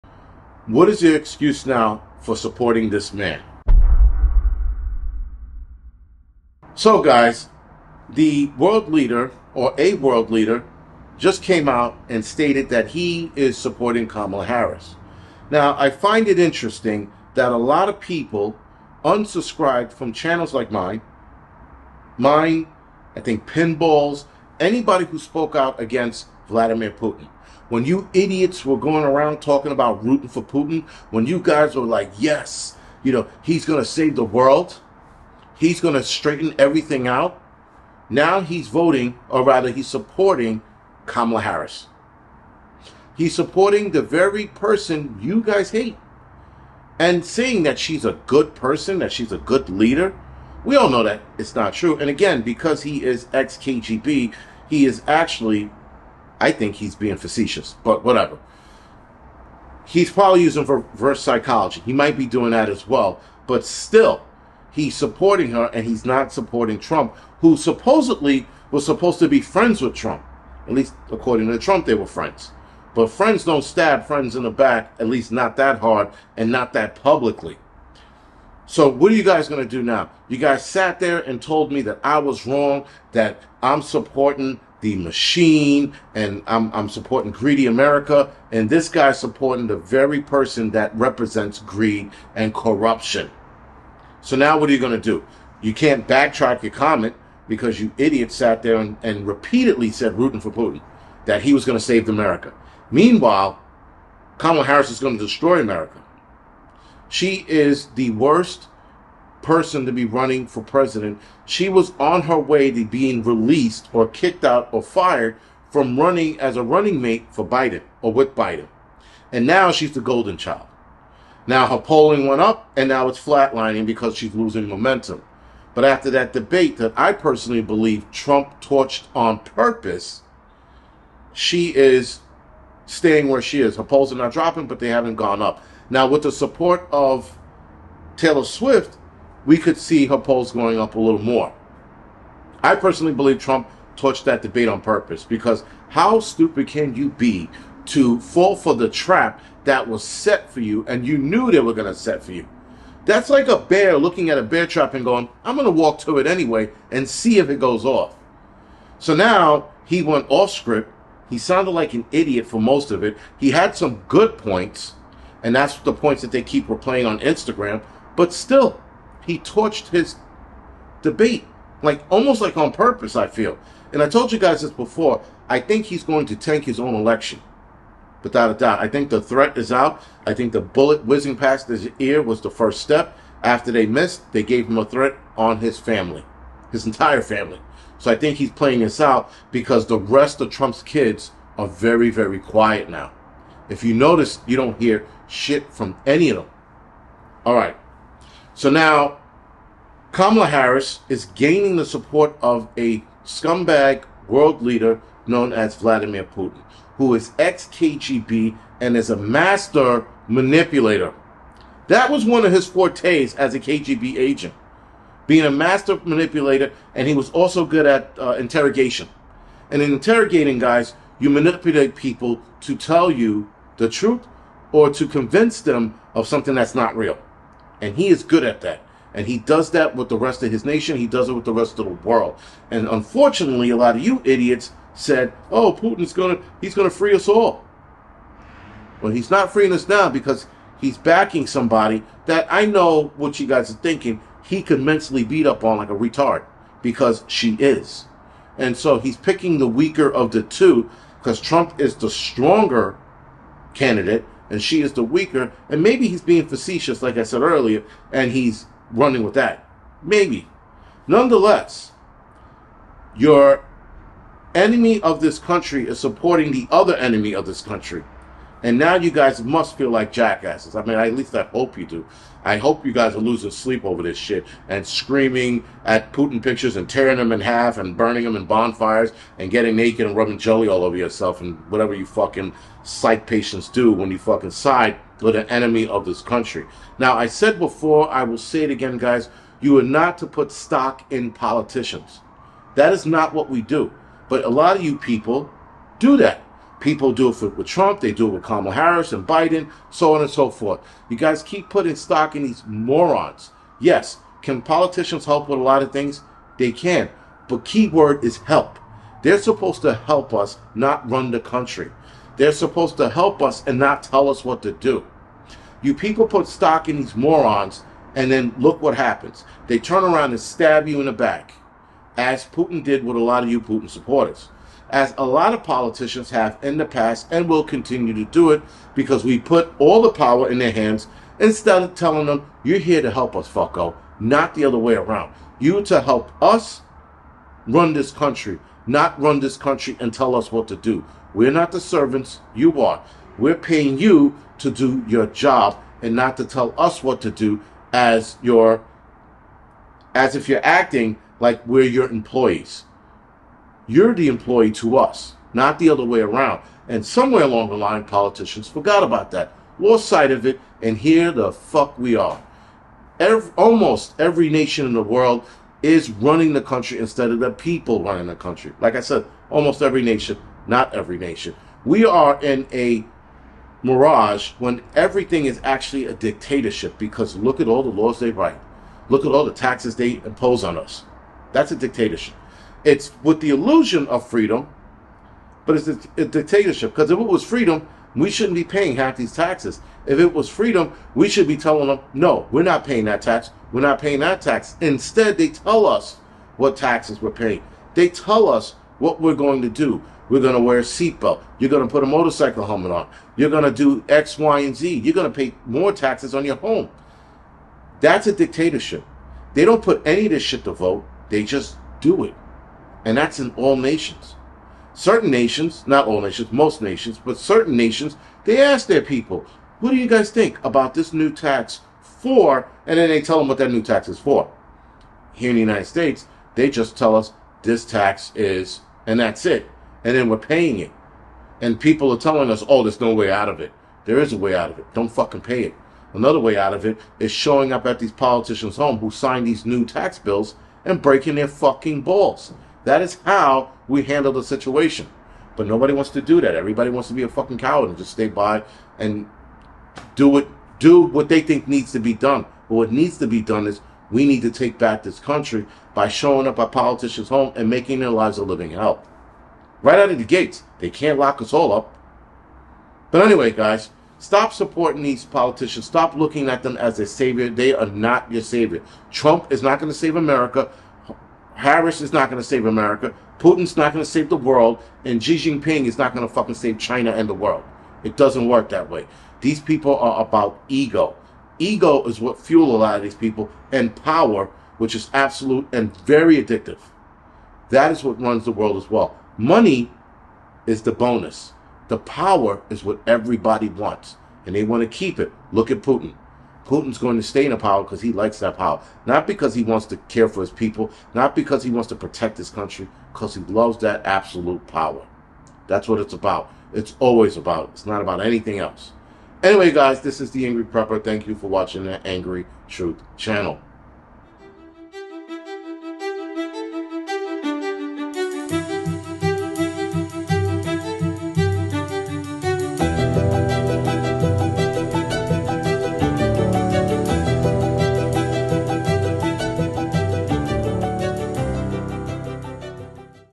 You Got Played...
On the Angry truth Channel, we are going to talk about how You Got Played by Putin.  Some people were in full support of him & now what are they are they going to do?  Let's see if they still support him the same.